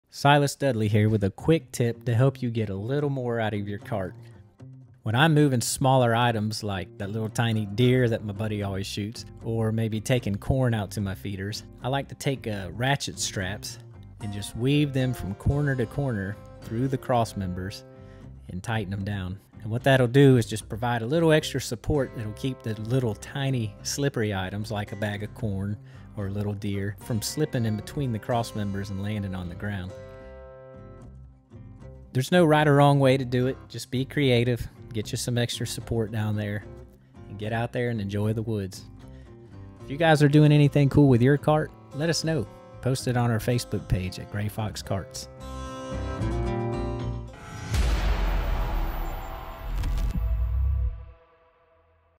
How To Carry Smaller Items In Your gray Fox Game Cart
Check out how to create a webbing on the bottom of your gray fox cart. This webbing will act as support for smaller items you will carry, such as corn bags, small deer, and other small items.